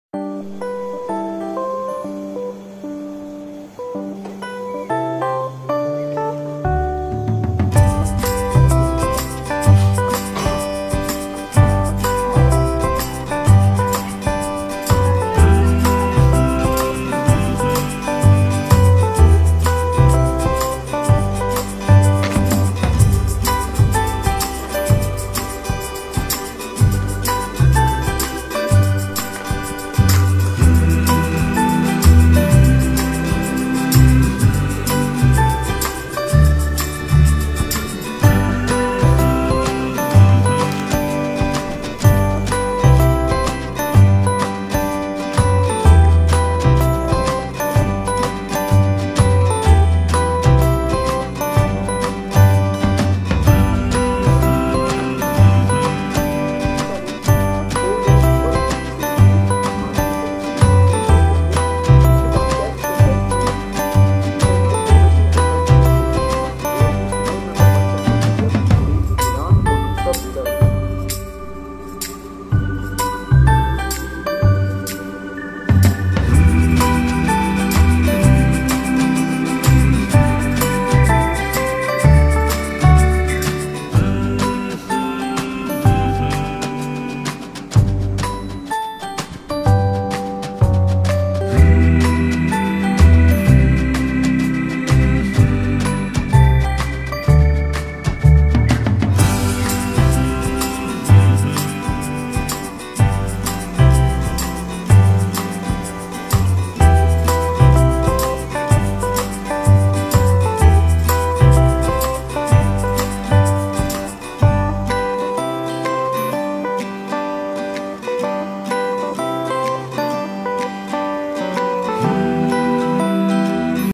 UPSC preparation from home||Aspirant study vlog || NO COACHING || Day in a life.
hi guys. i am a upsc aspirant aiming for 2024 upsc cse exam . i will share my study plan and routine with you guys.